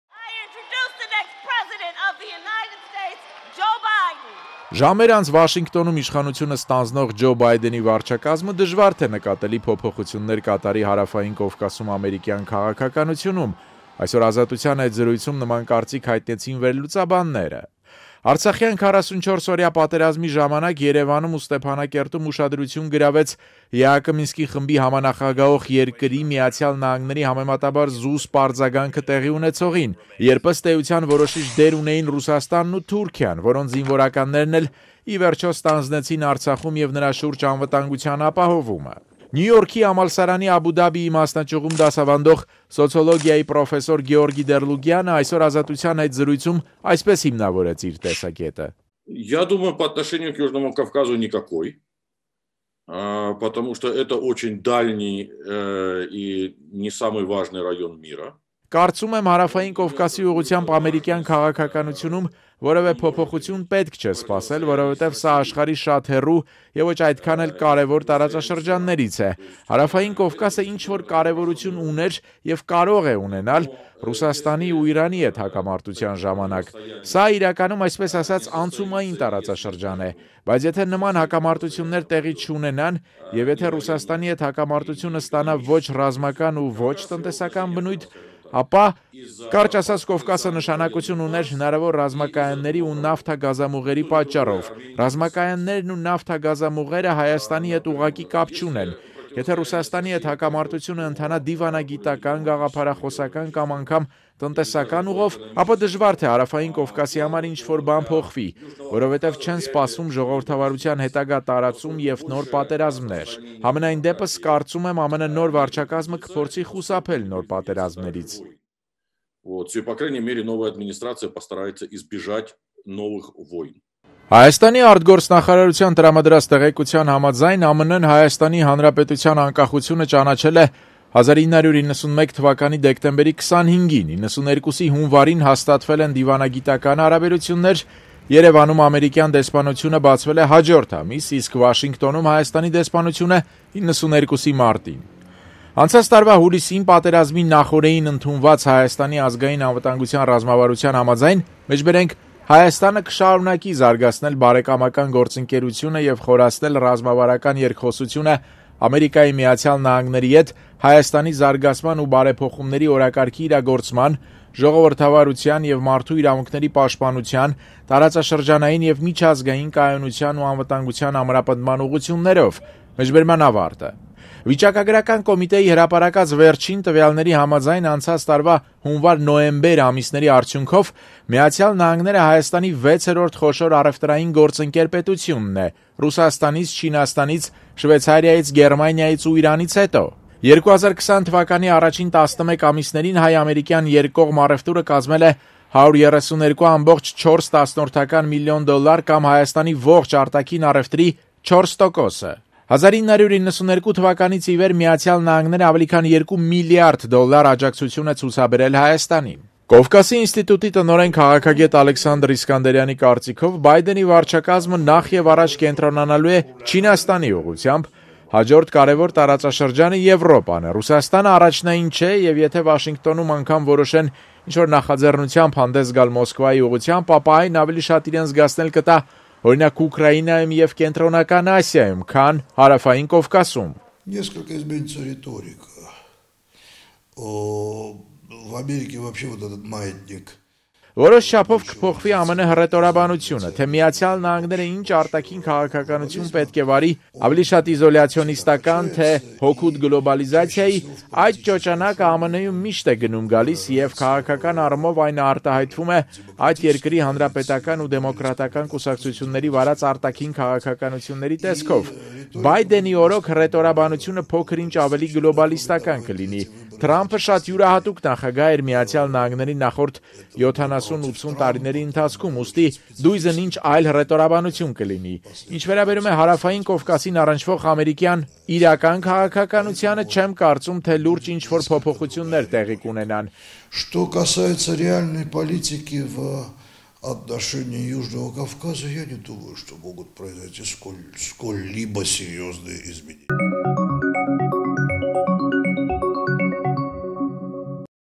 Բայդենի վարչակազմը նկատելի փոփոխություններ չի կատարի Հարավային Կովկասում ԱՄՆ քաղաքականությունում
Բայդենի վարչակազմը նկատելի փոփոխություններ չի կատարի Հարավային Կովկասում ԱՄՆ քաղաքականությունում. վերլուծաբաններ